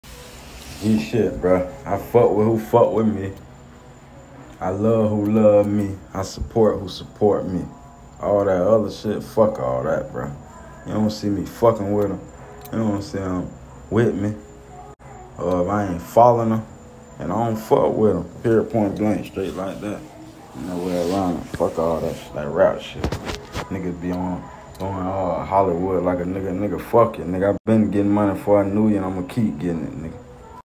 Pakman Jitt speaks on rappers acting Hollywood & not supporting him when he got features from them?
Pakman Jitt speaks on rappers going Hollywood & not supporting him when he got features from them?